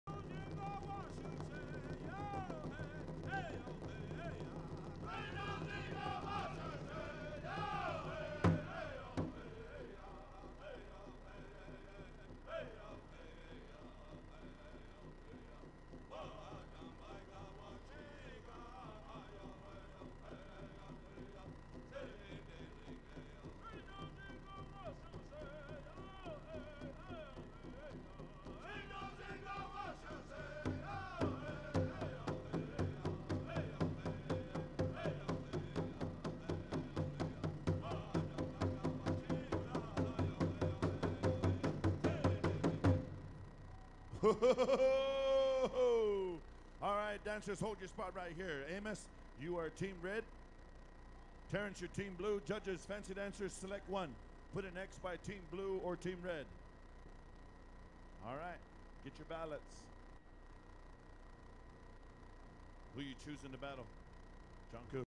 Men's Fancy Special (3) - Sioux Empire Pow Wow - PowWows.com
Men's Fancy Special by DJ Two Bears part 3
2017 Sioux Empire Pow Wow